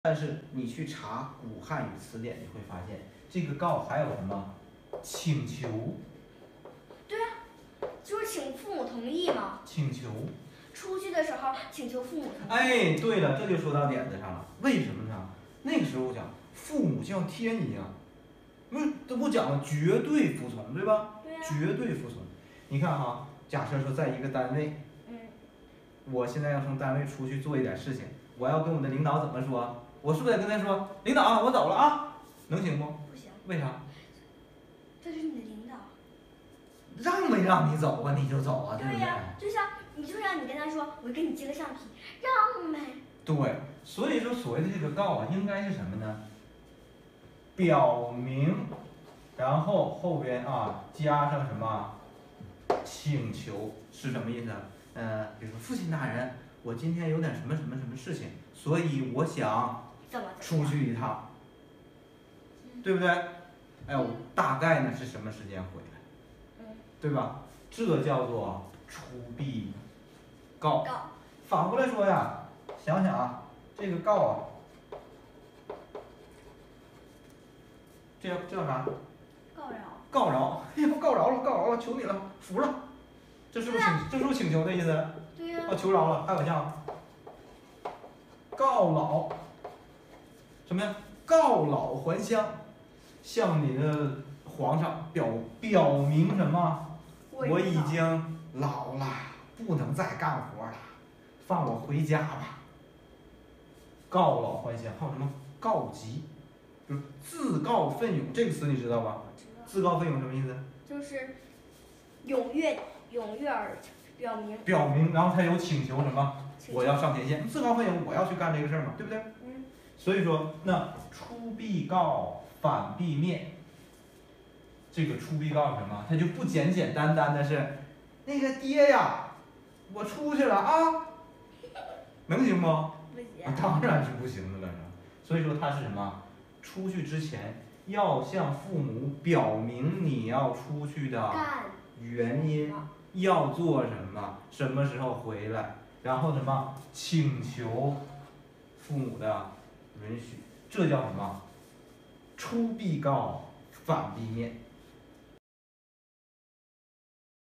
出门时跟父母打个招呼就叫“出必告”？那您把弟子规想简单了
個人觀點僅供參考
縱你閱人何其多，始終無人恰似我
每日更新分析古詩感興趣的點個訂閱謝謝大家